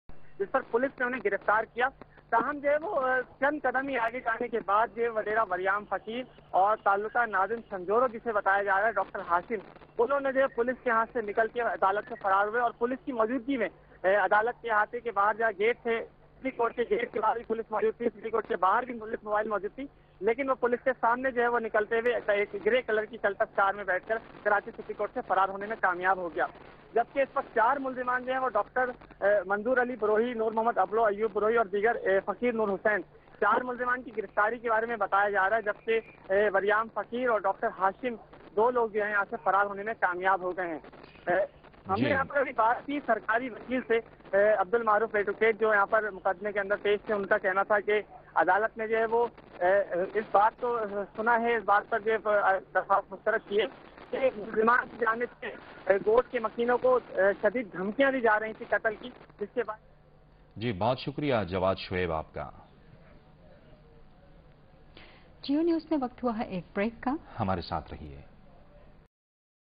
wadero waryam fakir
sangher ka wadera aur ek haari ke jang